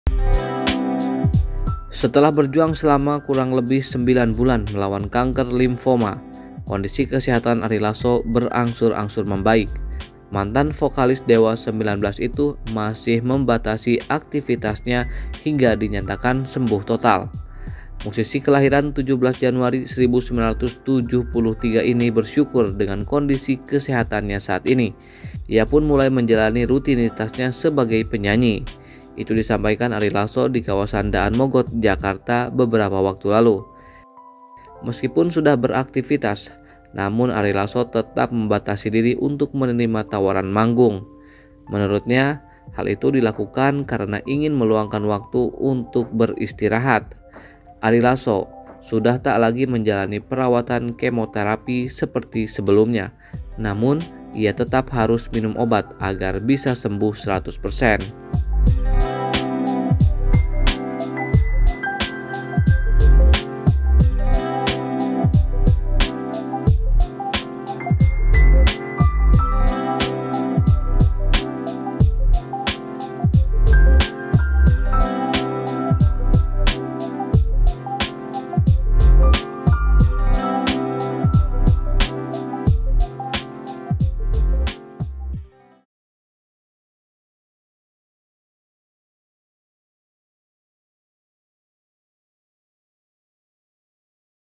Kondisi Kesehatan Membaik, Ari Lasso Masih Batasi Jadwal Manggung
Setelah berjuang selama kurang lebih sembilan bulan melawan kanker limfoma, kondisi kesehatan Ari Lasso berangsur-angsur membaik. Namun, mantan vokalis Dewa 19 itu masih membatasi aktivitasnya hingga dinyatakan sembuh total.

BACA JUGA: Jadwal Konser Dewa 19 2023, Ahmad Dhani Bawa Formasi Lengkap